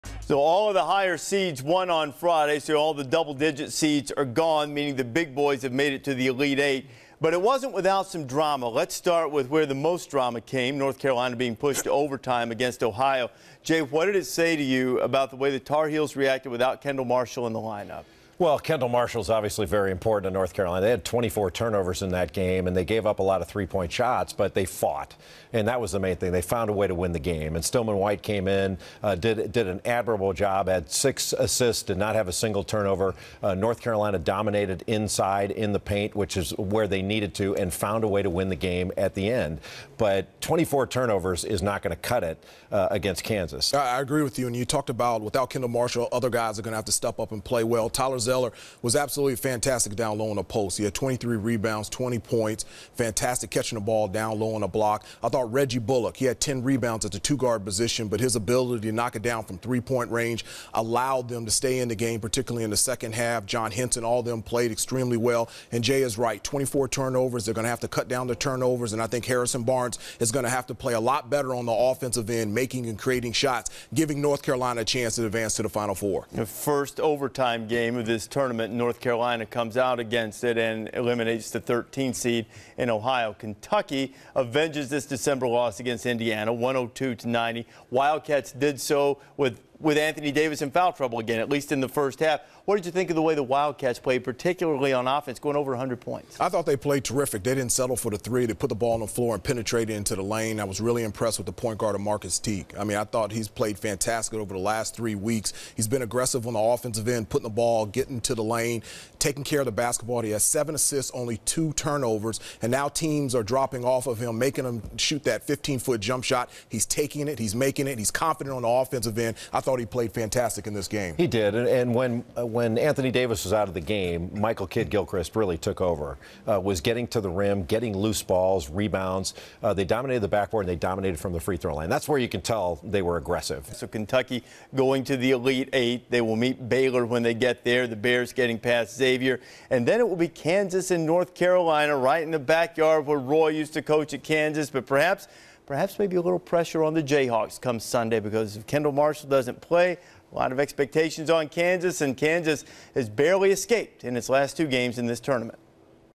CBB: Top Seeds Advance On Friday
Hubert Davis and Jay Bilas break down North Carolina's win over Ohio, and Kentucky's win over Indiana.